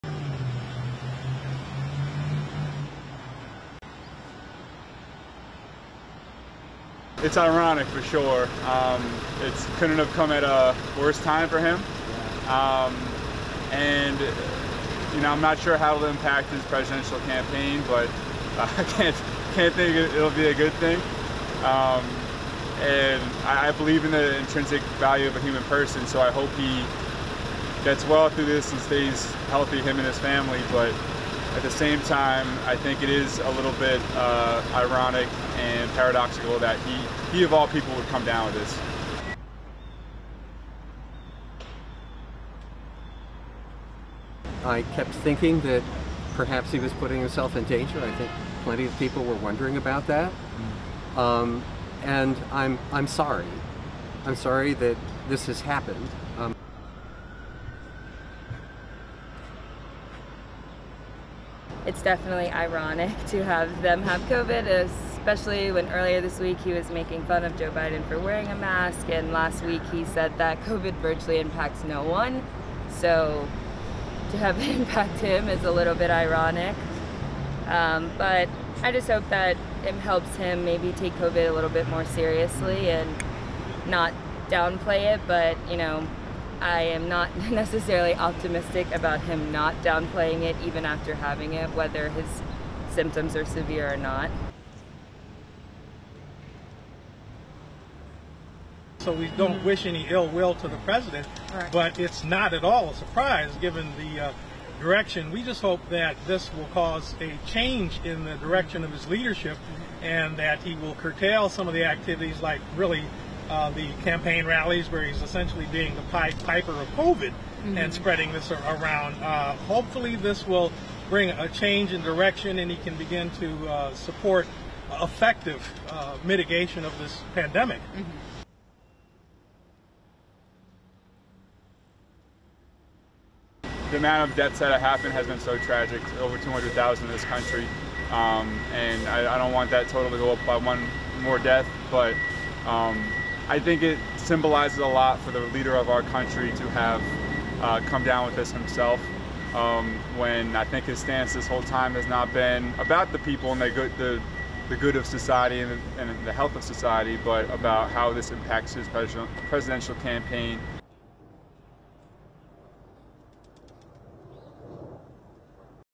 Voters react to Trump virus test results
(2 Oct 2020) Washington, D.C. residents and visitors reacted to the stunning news Friday that President Donald Trump and first lady Melania Trump have both tested positive for coronavirus.
The White House said that President Donald Trump was suffering “mild symptoms” of COVID-19, making the announcement after he returned from an evening fundraiser without telling the crowd he had been exposed to an aide with the disease that has killed a million people worldwide.
The announcement that the president of the United States and first lady Melania Trump had tested positive, tweeted by Trump shortly after midnight, plunged the country deeper into uncertainty just a month before the presidential election.
However, White House aides said he was in good spirits and working in quarantine in the family quarters.
Trump has spent much of the year downplaying the threat of a virus that has killed more than 205,000 Americans.
His diagnosis was sure to have a destabilizing effect in Washington and around the world, raising questions about how far the virus has spread through the highest levels of the U.S. government.
Hours before Trump announced he had contracted the virus, the White House said a top aide who had traveled with him during the week had tested positive.